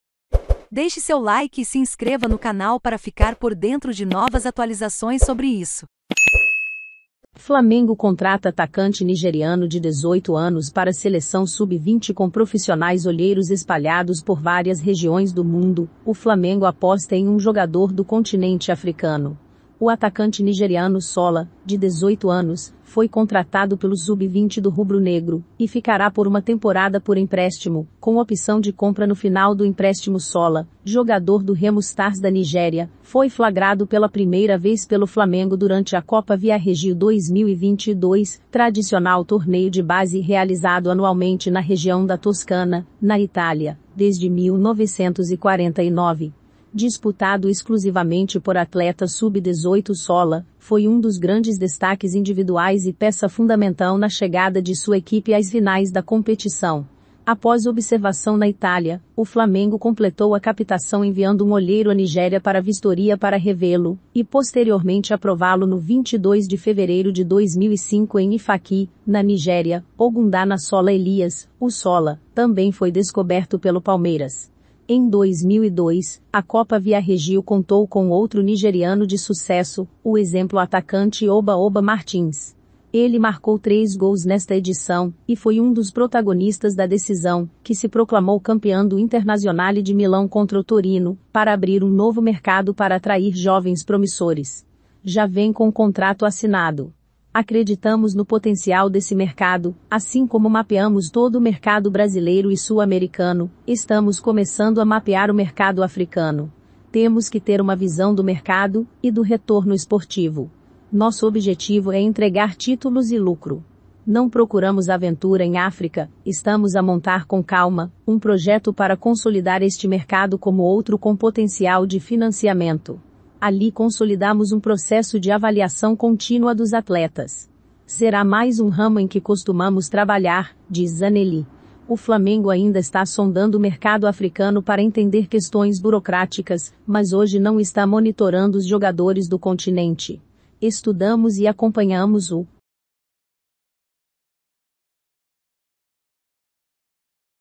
FLAMENGO CONTRATA ATACANTE NIGERIANO DE 18 ANOS PARA A SELEÇÃO SUB 20
Shola. jogador do Remo Stars da Nigéria. foi flagrado pela primeira vez pelo Flamengo durante a disputa pela Copa Viarregio 2022. tradicional torneio de base realizado anualmente na região da Toscana. na Itália. desde 1949.